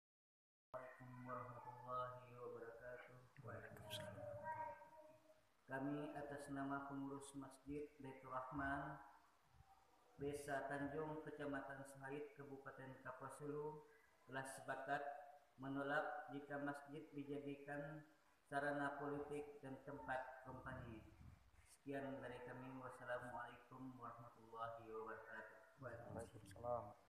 Video Deklarasi Pilkada Damai dan Anti Berita Hoax Masyarakat Kabupaten Kapuas Hulu
Deklarasi Pilkada Damai dan anti berita HOAX serta mendukung penindakan terhadap penyebaran HOAX oleh POLRI Yg disampaikan oleh Pengurus Mesjid BAITURAHMAN Desa. Tanjung Kec. Suhaid kab. Kapuas Hulu